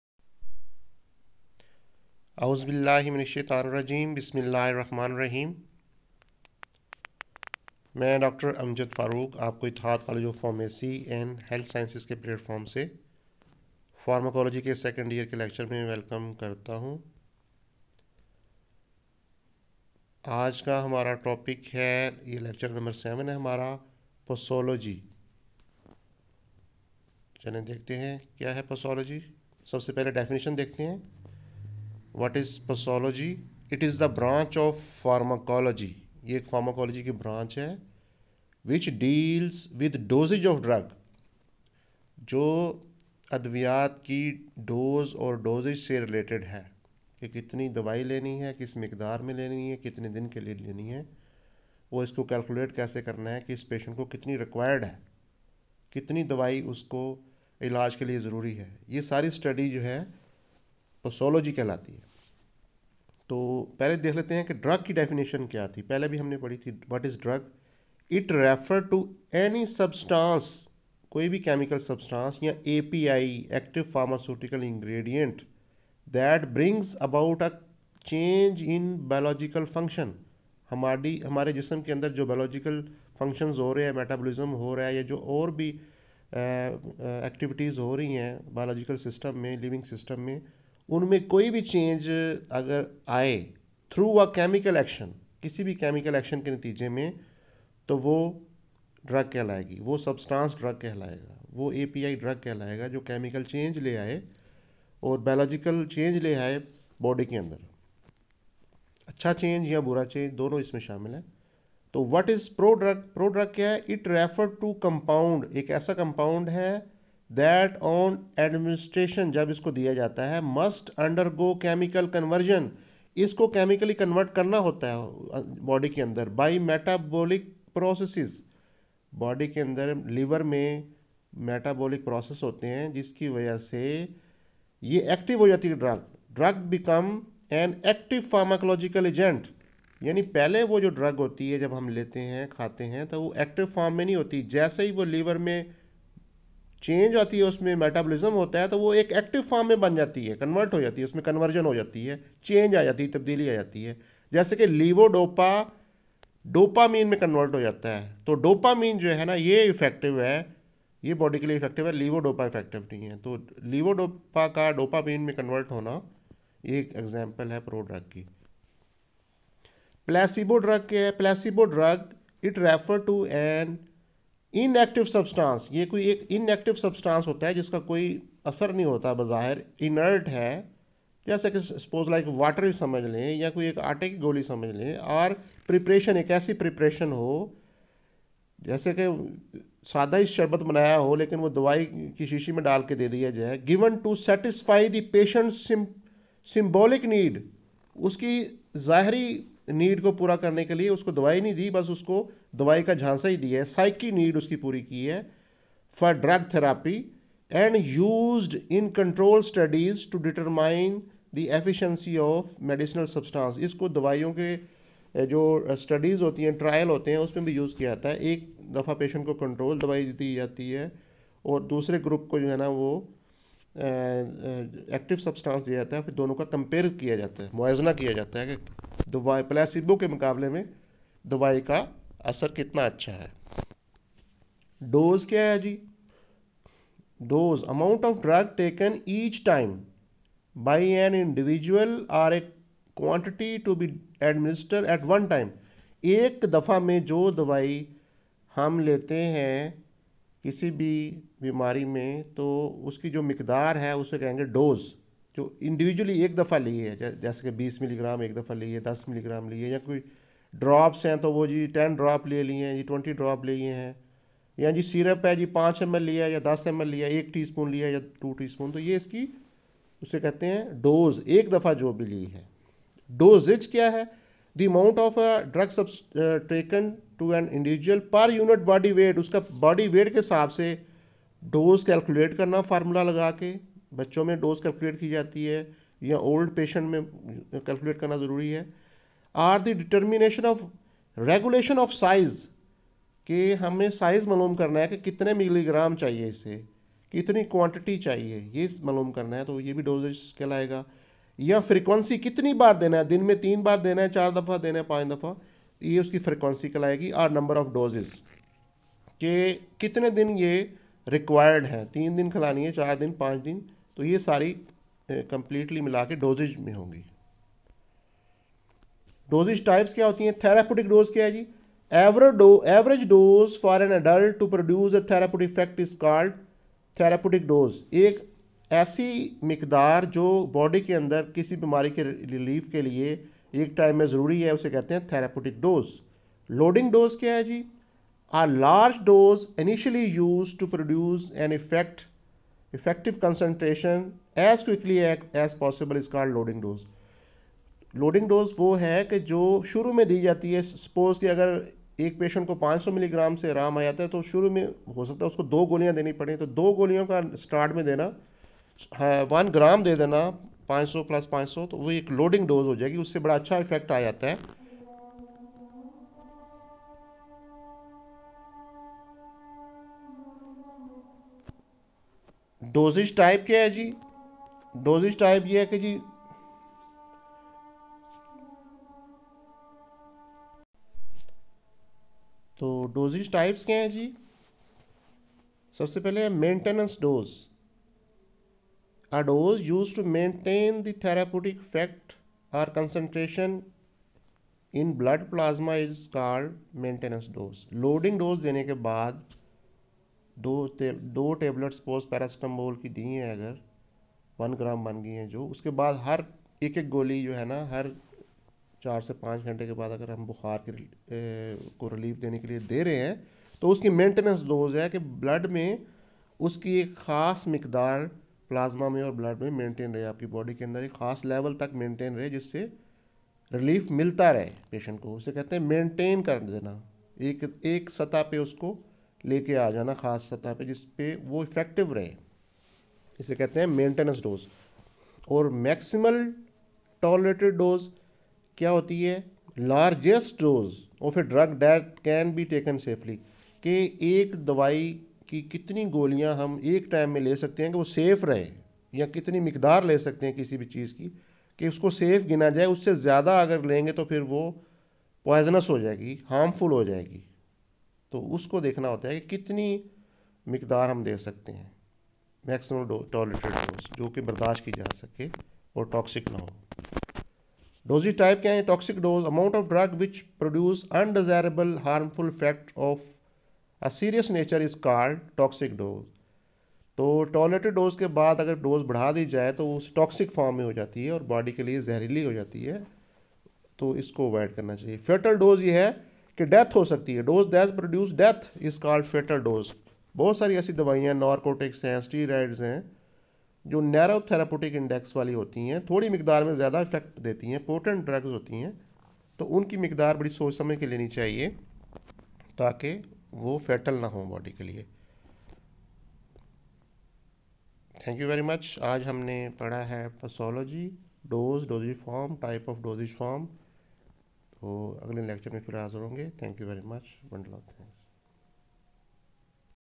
"Demystifying Posology: A Comprehensive Guide to Dose and Dosage Form"
Ittehad college of pharmacy & health sciences Rahim yar khan

Welcome to our comprehensive guide on posology, where we unravel the mysteries surrounding dose and dosage form. In this enlightening video, we dive deep into the world of pharmaceuticals and explore the crucial aspects of medications that directly impact their effectiveness.